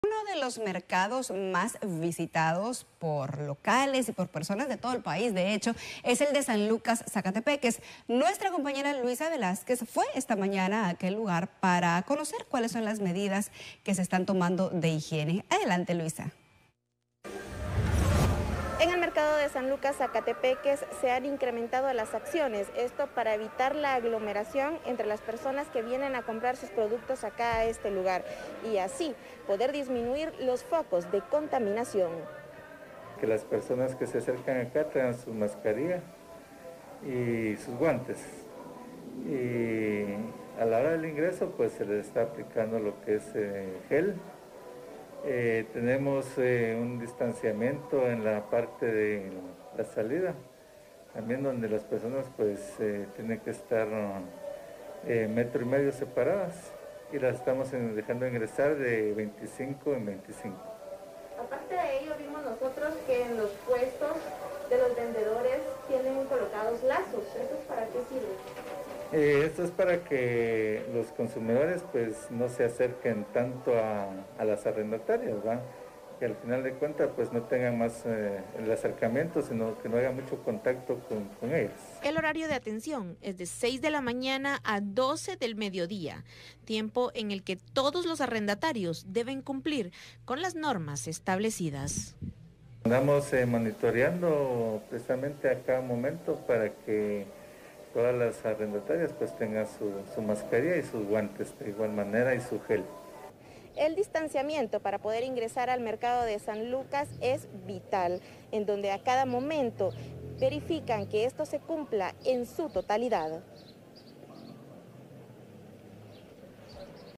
Implementan medidas de higiene en San Lucas Sacatepéquez
Locatarios y comerciantes del Mercado de San Lucas Sacatepéquez han incrementado las medidas de higiene para evitar contagios de COVID-19 en el lugar. Clientes deben ingresar con mascarilla y aplicarse alcohol gel en el ingreso al mercado